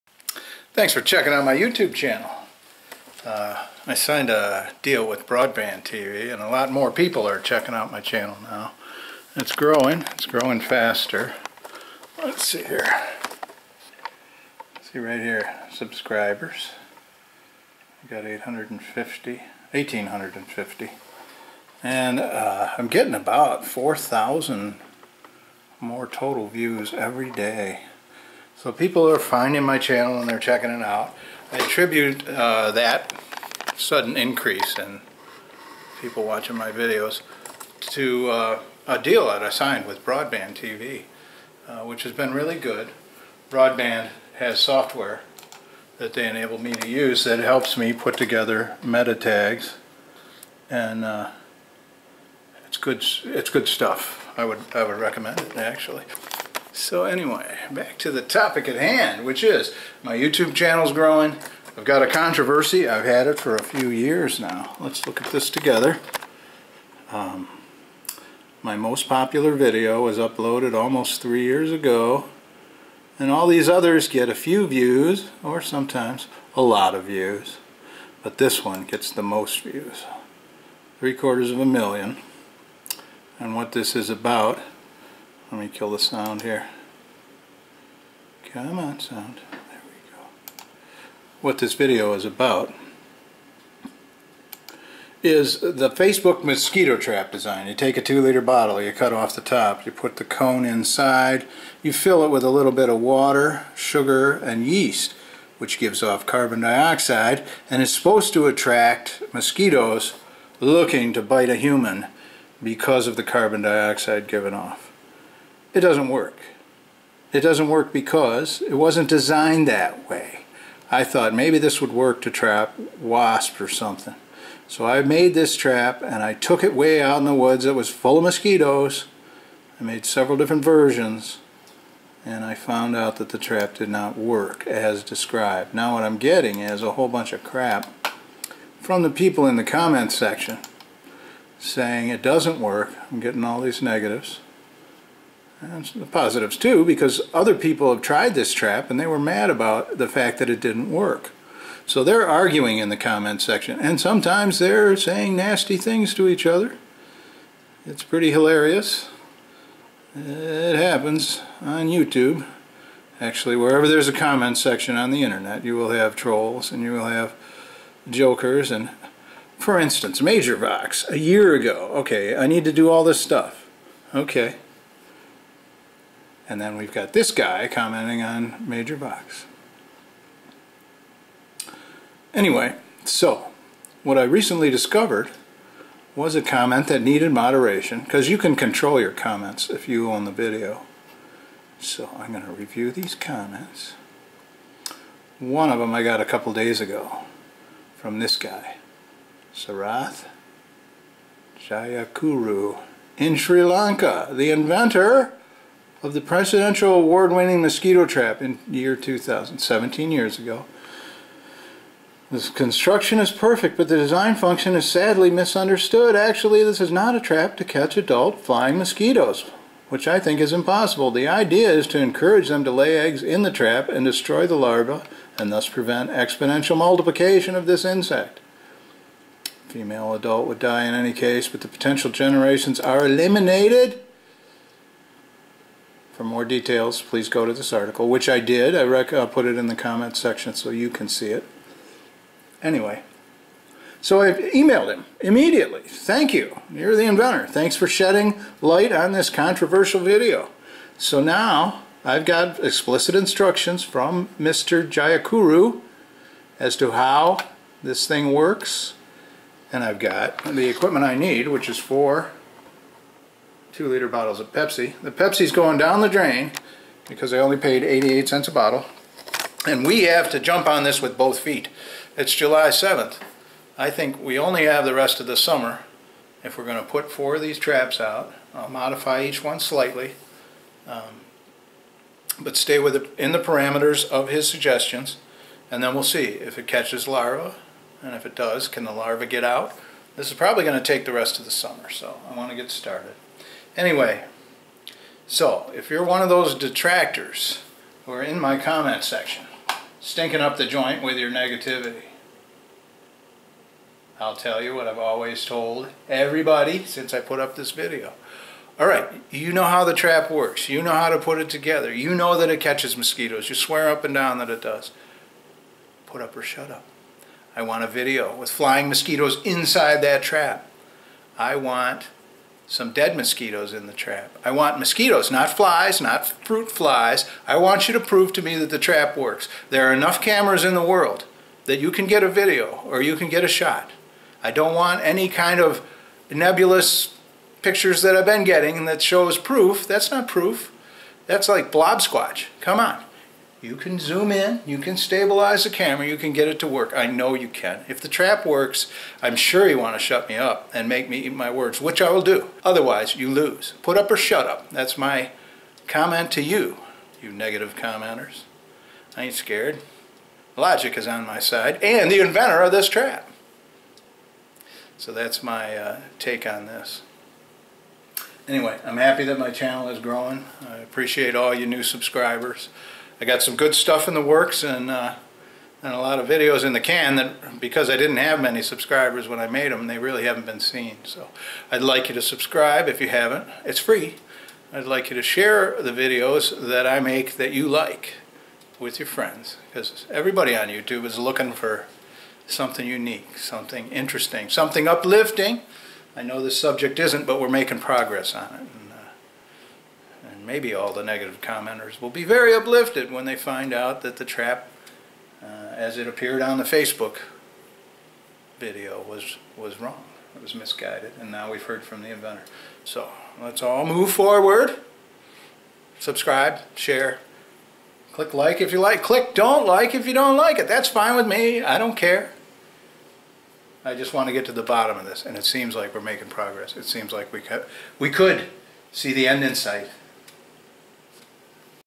My Most Controversial Video: New Info from The Inventor of the Mosquito Trap
I kept seeing the same posts on Facebook about how to make a mosquito trap with a 2 liter bottle. I never saw any posts that showed trapped mosquitoes, so I decided to make a video of me testing the trap. It didn't work. I posted my video and now have 750,000 views and lots of negative comments from viewers who say the trap works, I just made it wrong. Other viewers who've made the trap like my video and argue with the negative commentors, filling my comment section with expletives. Not the Grandkid friendly Youtube Channel I had in mind when I started this adventure. My most popular video is also my least favorite. People can't we all just get along?
Well, now we can. The haters can admit they're wrong for two reasons:
1. In the three years this video has been up, not one person commenting that the trap works has provided proof. The world is now full of cameras. Provide a pic or video of mosquitoes caught in the trap or I call Balderdash.
2. I got a message from the inventor of the mosquito trap design. He says the trap is not designed to catch adult mosquitoes, but to trap the larvae that hatch from the eggs that adult mosquitoes lay in the trap. It uses plain water, not sugar and yeast to generate carbon dioxide. People were making stuff up and posting it on facebook, not knowing what they were talking about.
I'm making 4 traps according to the inventor's specs and will make a followup video of the traps' performance, but already we know that the trap is not designed to catch adult mosquitoes.